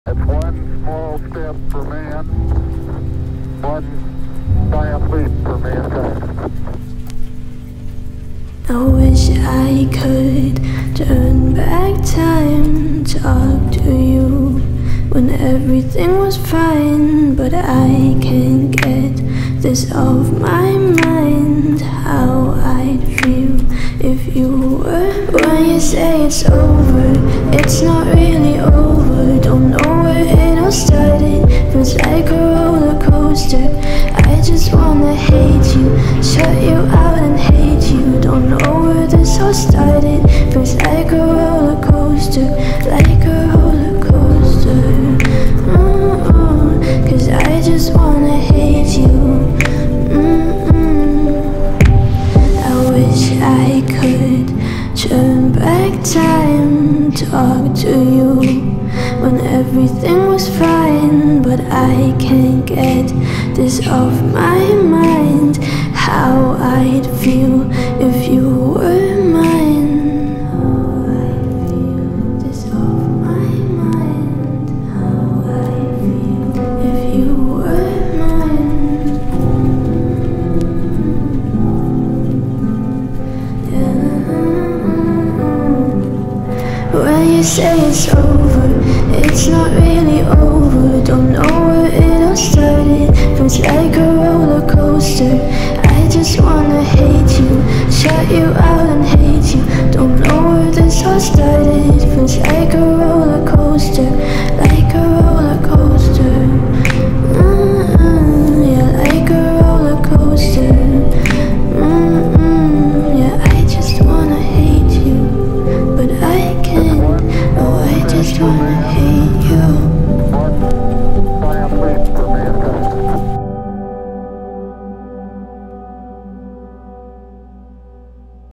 rollercoaster feelings ughh
hiii :)
i fully made this lil song this morning in like 3 hours so it's faaaar from perfect
but i decided to put sm visuals with it & post it on yt cuz i haven't posted on here in a while...
i hope u like this onee

ilyyy

💕thanku💕